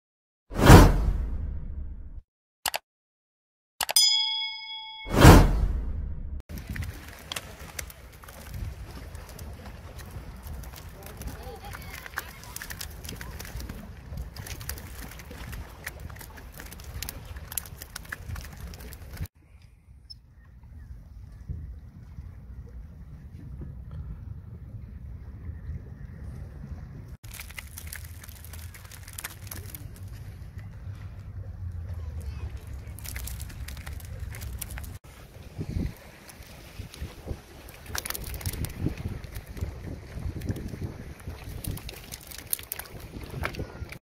12 Makrell på samme kast - Flott Lyr - Oslo
Fikk en stor Lyr og masse makrell denne dagen